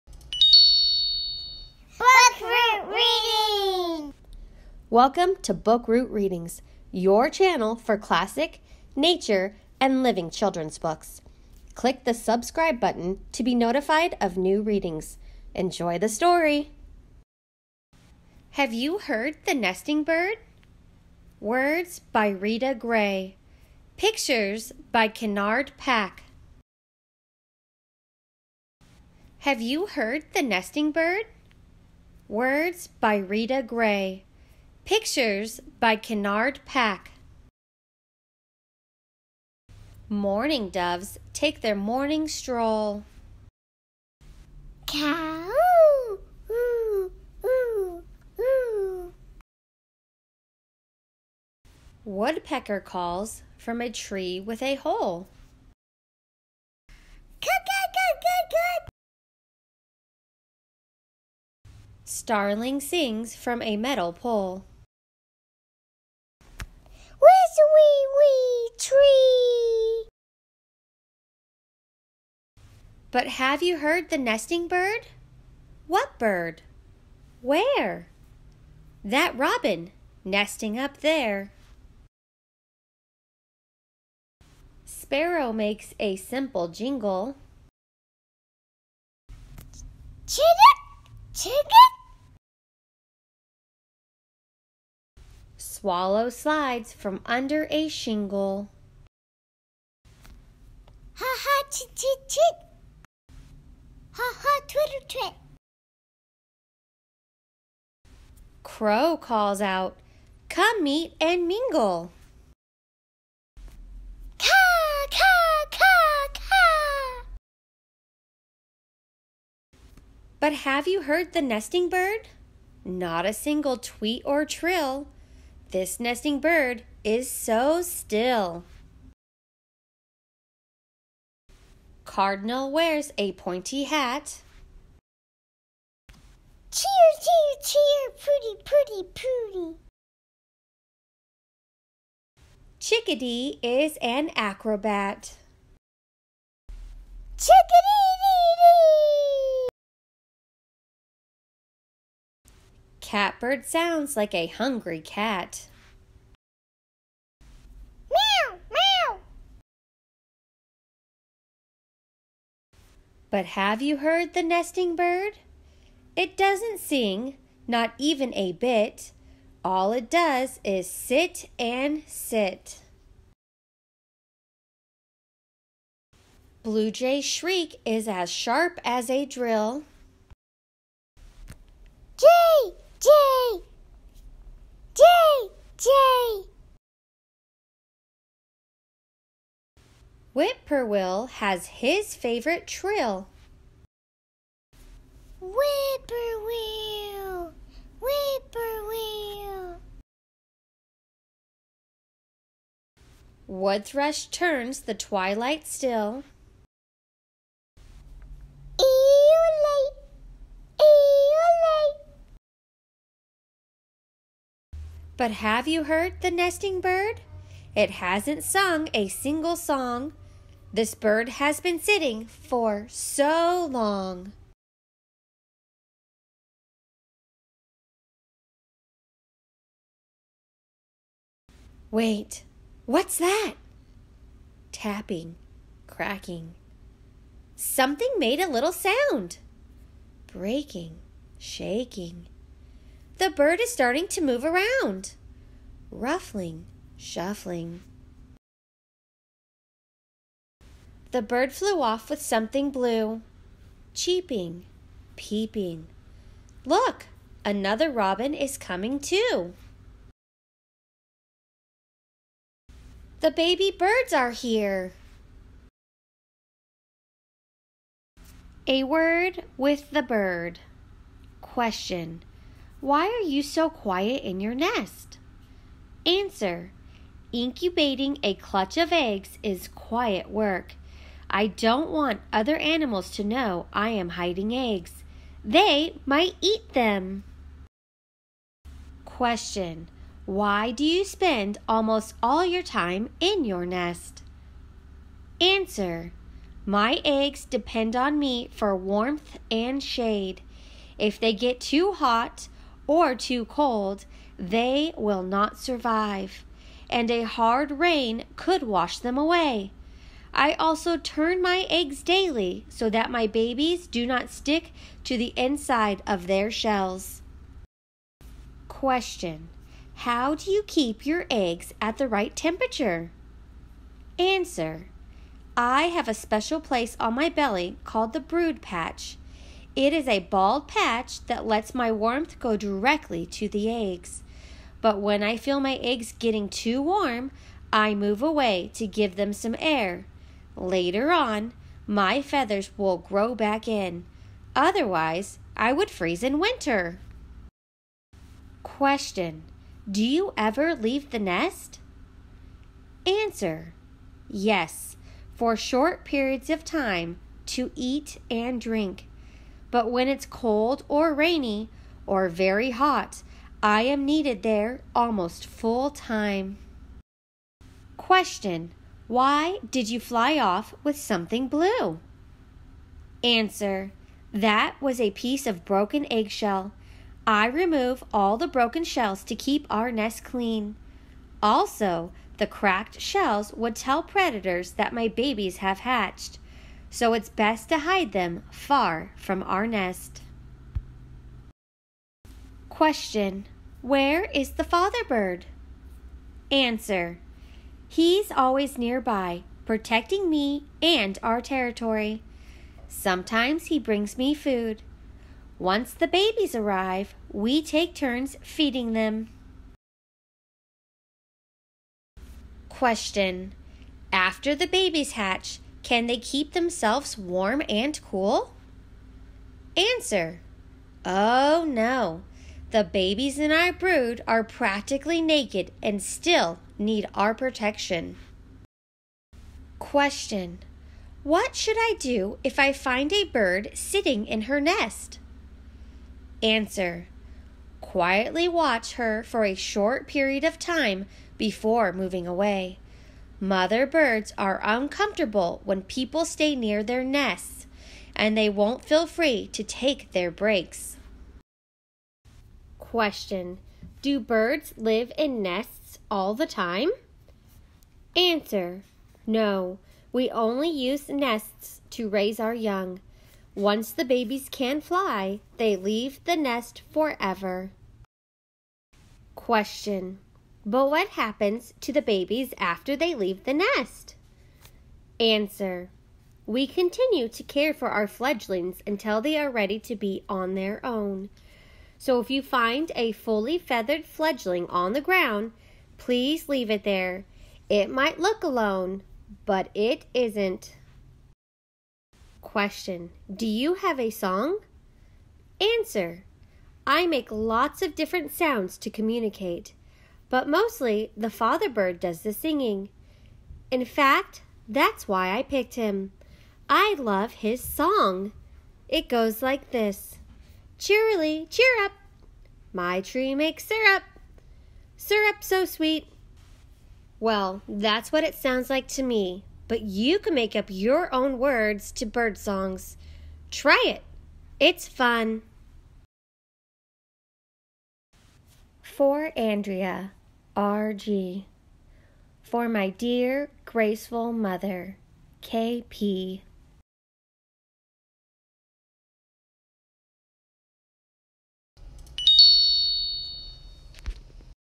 Kids Book Read Aloud: Have You Heard the Nesting Bird? Words by Rita Gray, Pictures by Kenard Pak
Do you know how to make the sounds of nesting birds? Follow along with this storytime to hear our versions. What do your bird calls sound like?

If you've found a new favorite I encourage you to get a copy of your own, either through purchase or from the library, to read with your favorite people again and again!

If you want to hear how the birds really sound, is a great resource!

If you want to help nesting birds, here is the Audubon's site that lists what materials are safe for birds